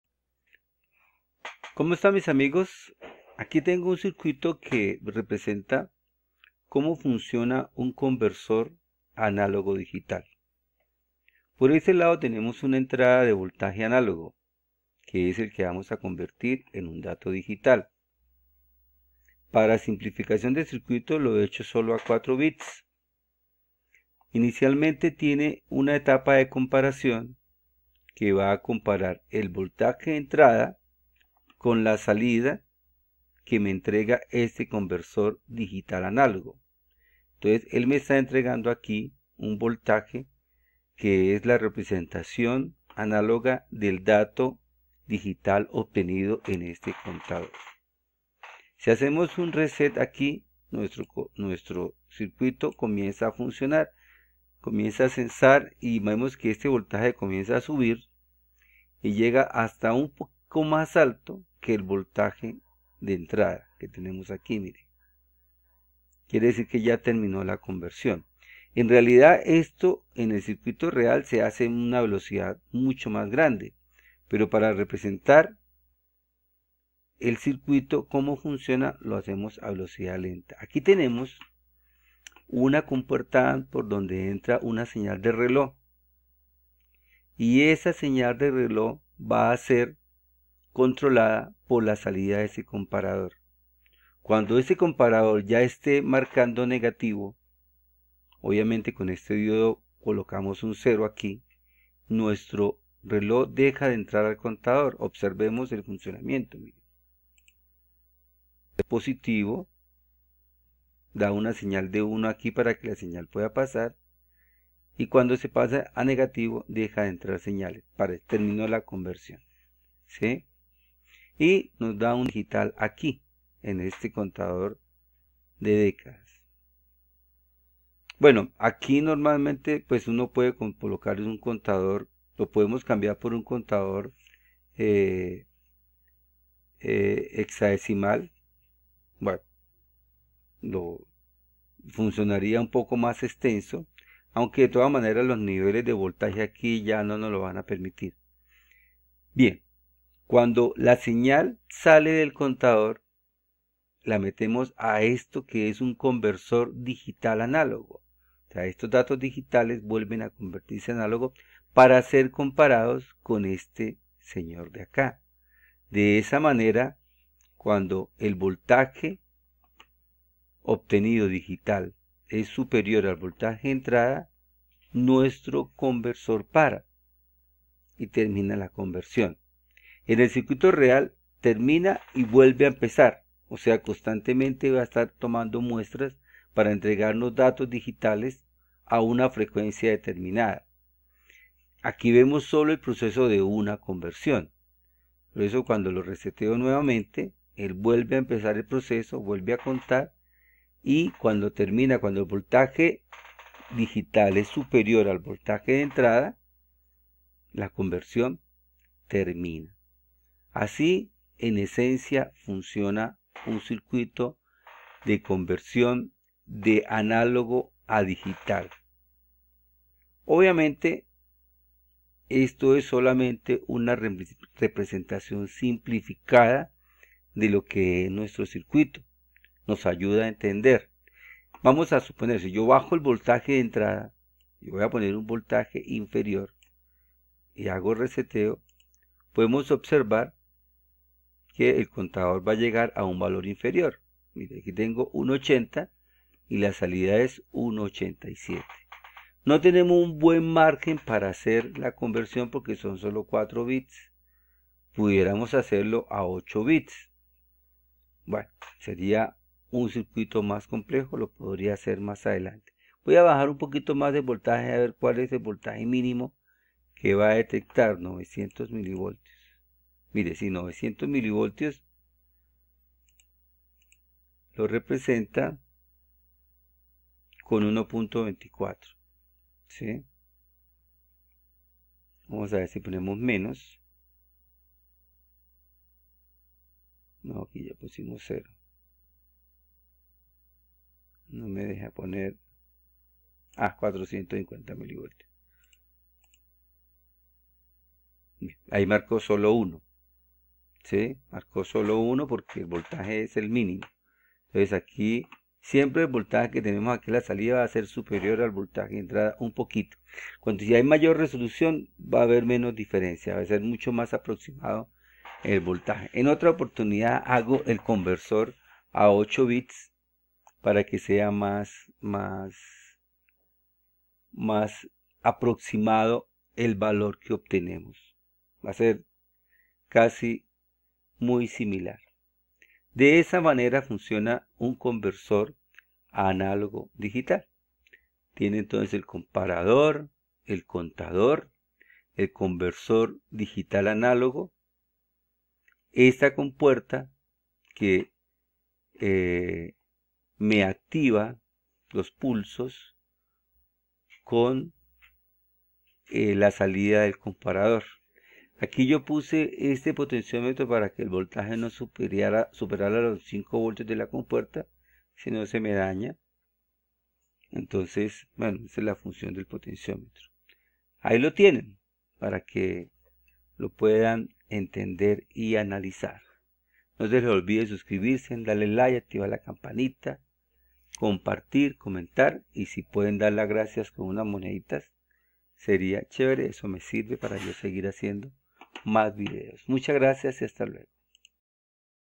conversor analogo digital simulado en cocodrile
en estse video vemos un conversor analogo digital simulado en tinkercad